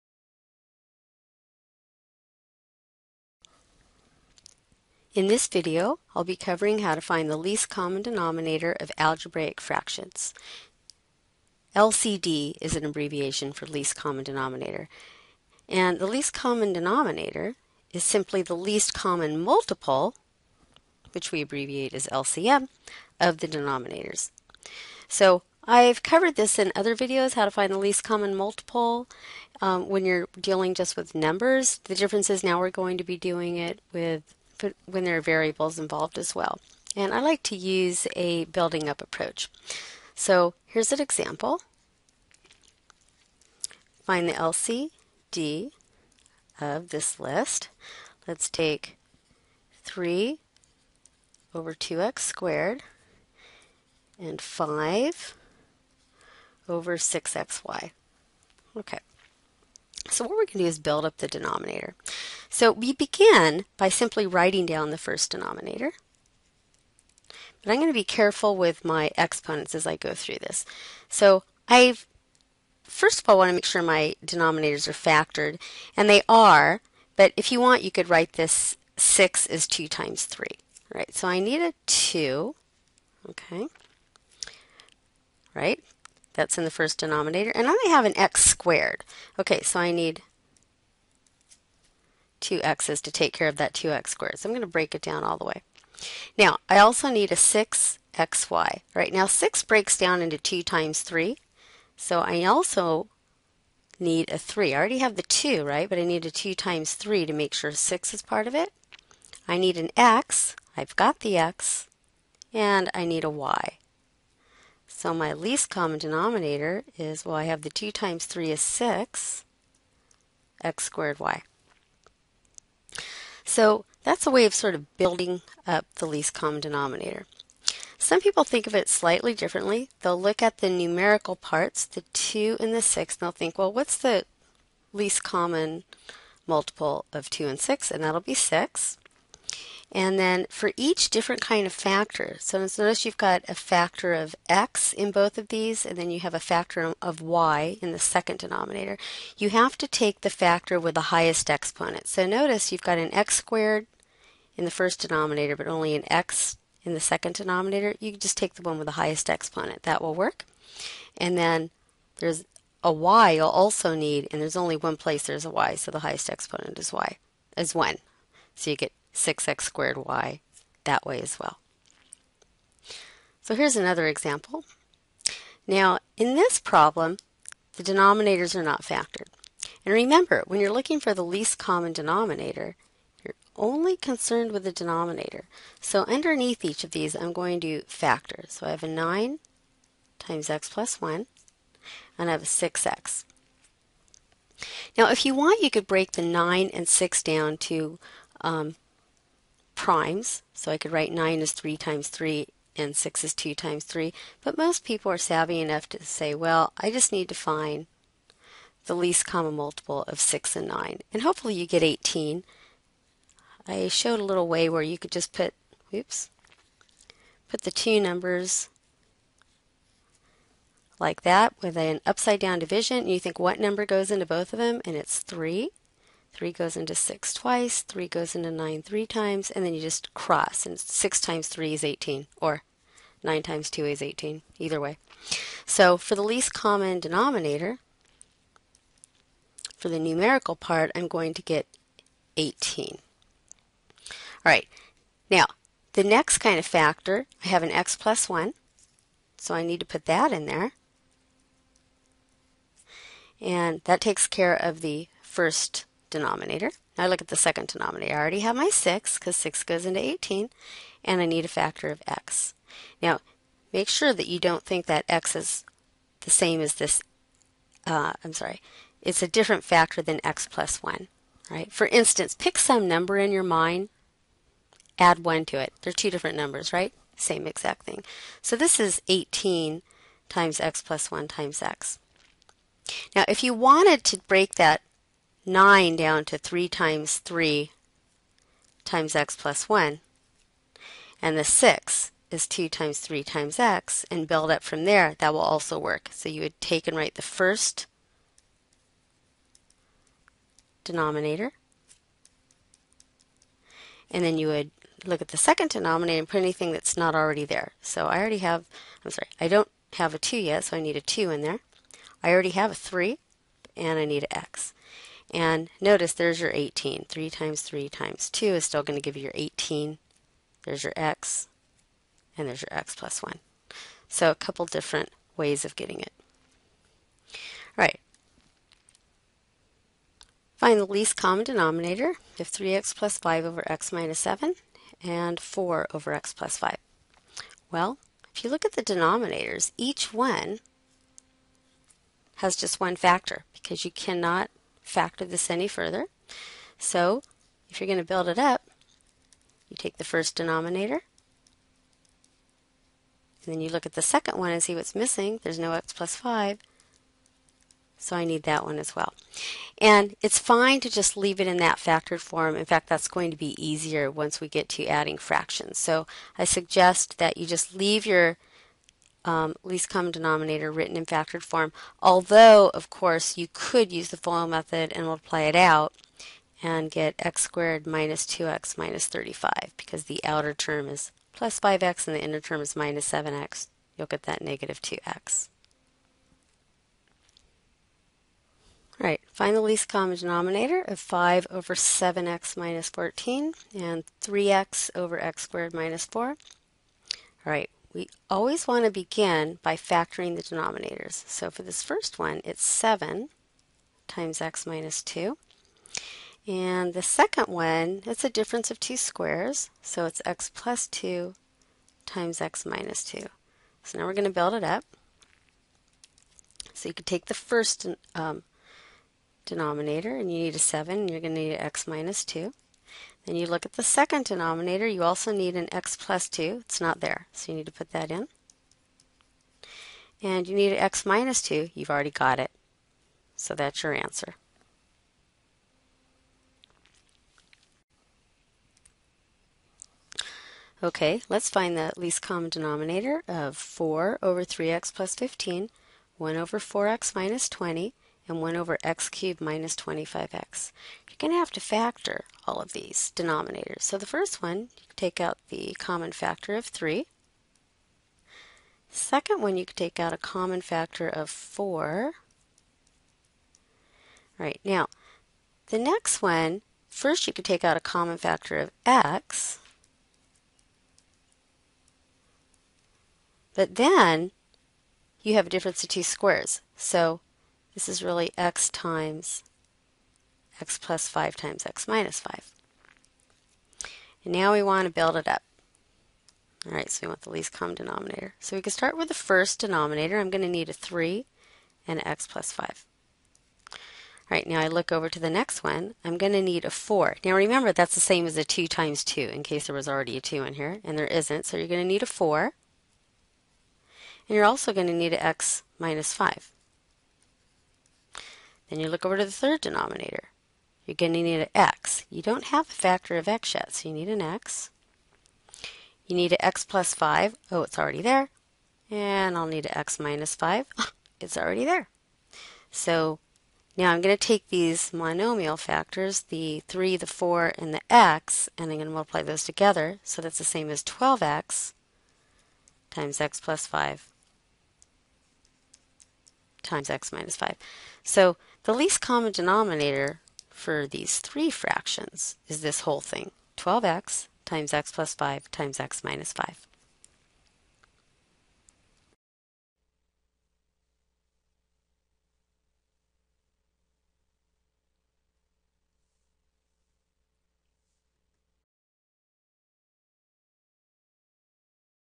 Finding the LCD of rational expressions
This video shows a method for finding the least common denominator of rational expressions.  I've posted at least 2 other videos with methods, explanations, and examples for finding the LCD. Video by Julie Harland